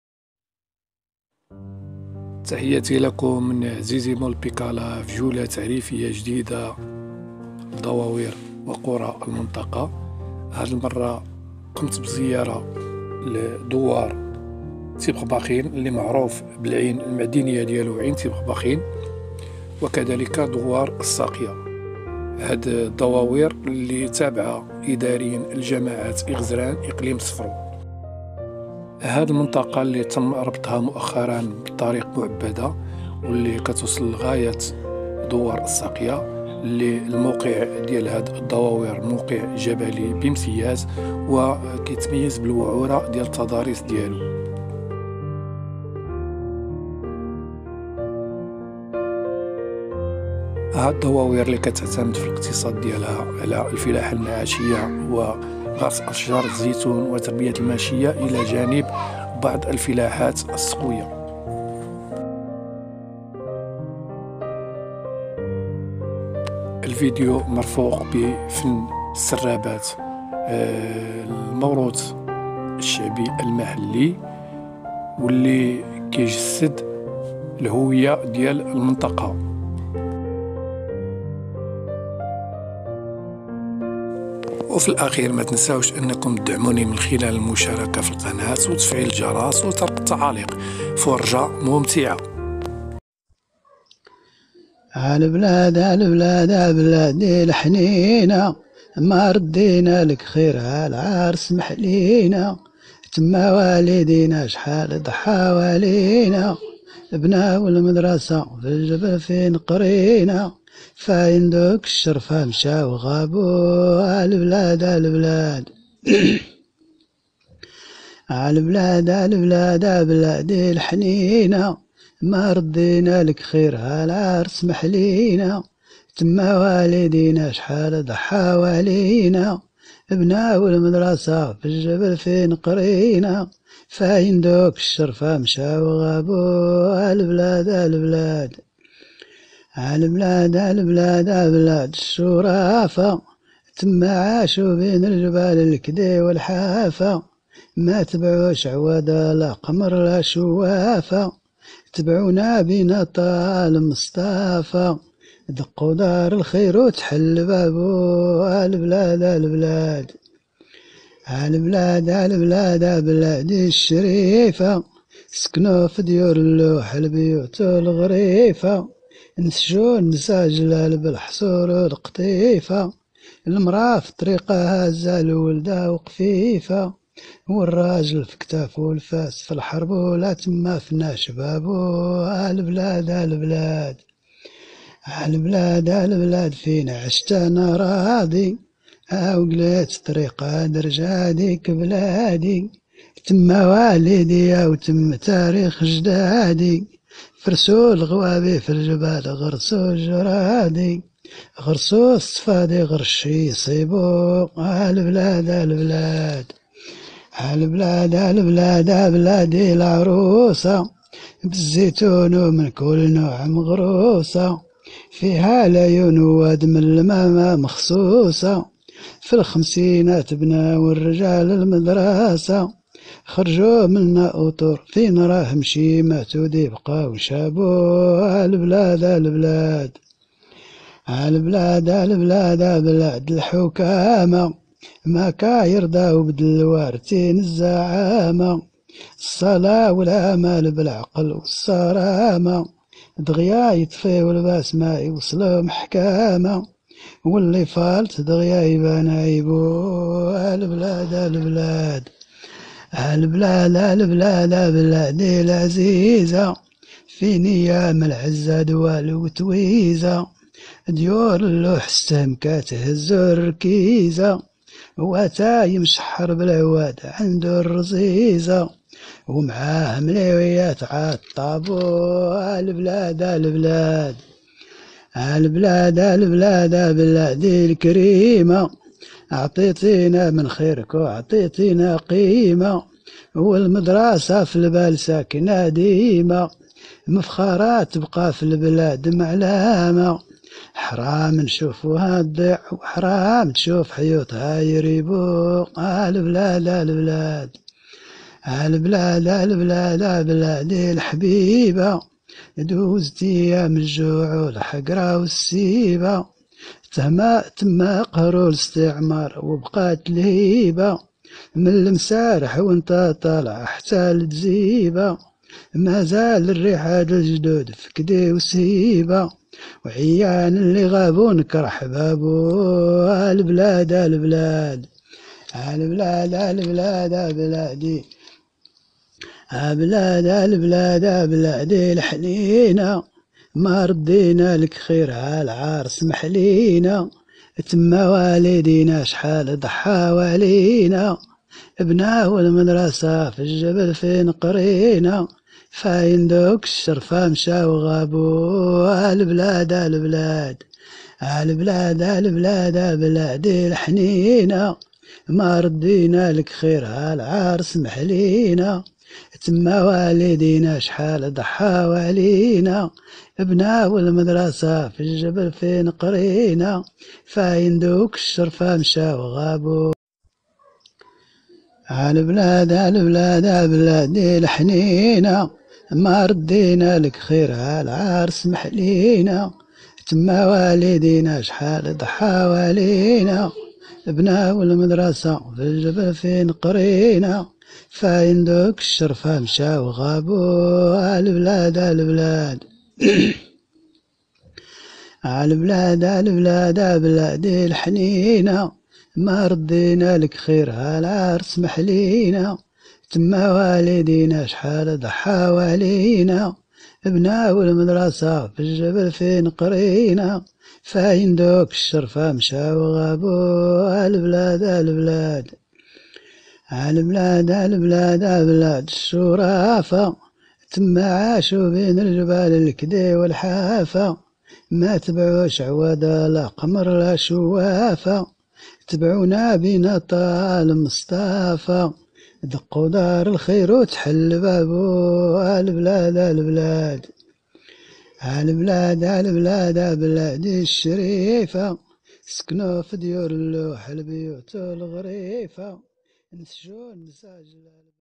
مشينا زرنا دوار تبخباخين ودوار الساقية،جماعة إغزران /إقليم صفرو/.
منطقة دوار الساقية وتبخباخين تتميز بتضاريسها الصعبة والجبلية ..